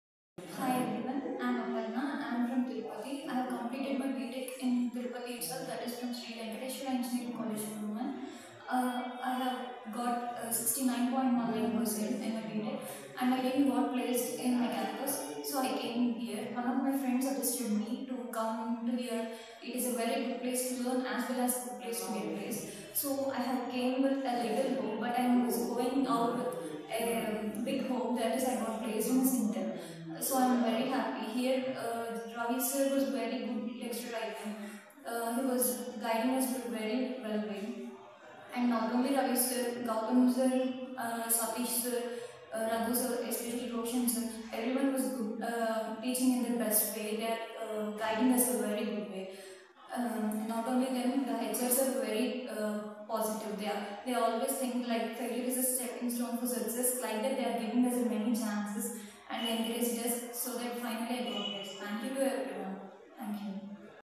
Jspiders Old Airport Road
Congratulations Aparna got placed sharing her experience about Jspiders